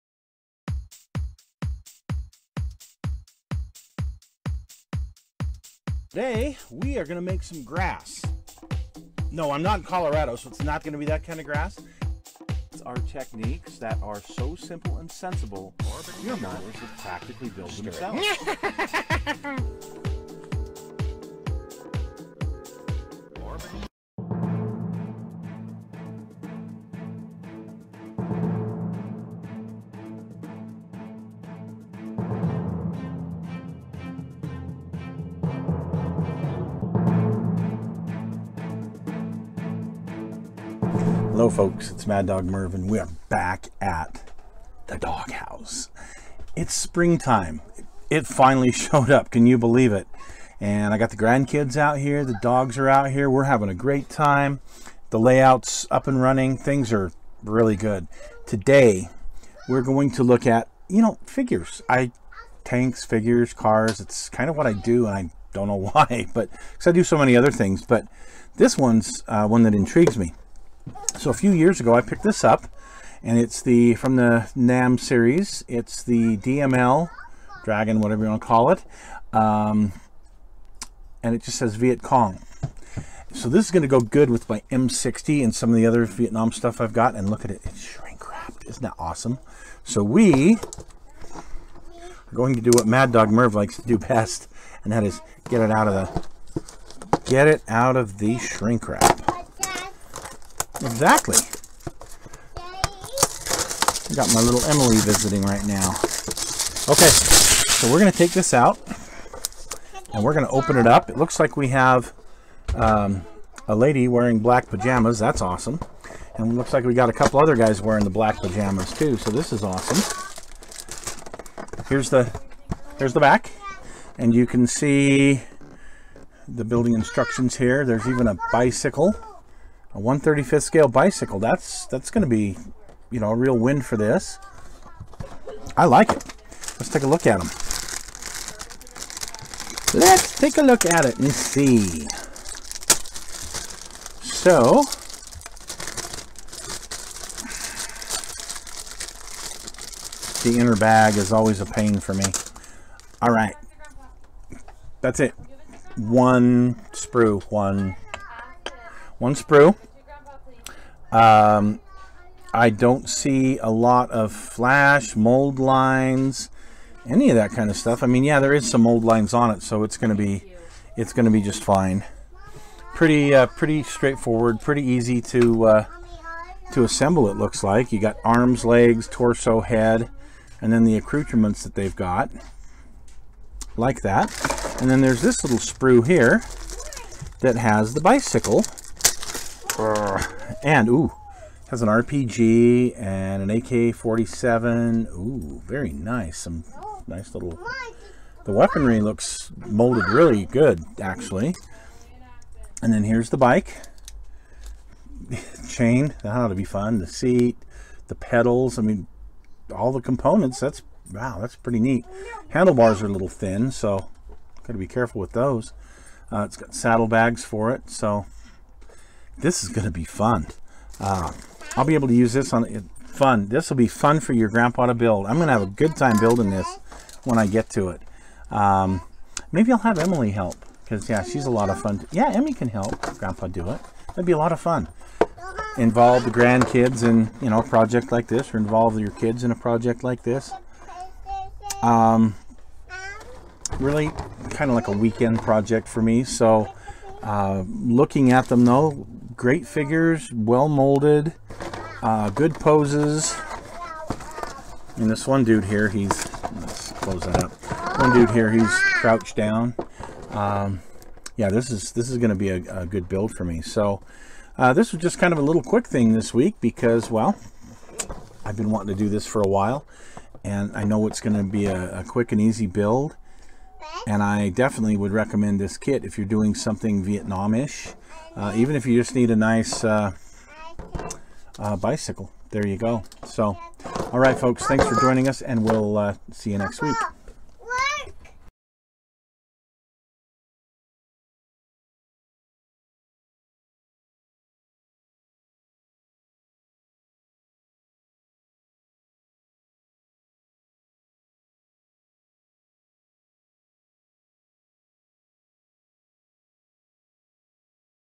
DML 1/35 scale Viet cong
This week I review the DML 1/35 scale Viet cong figure set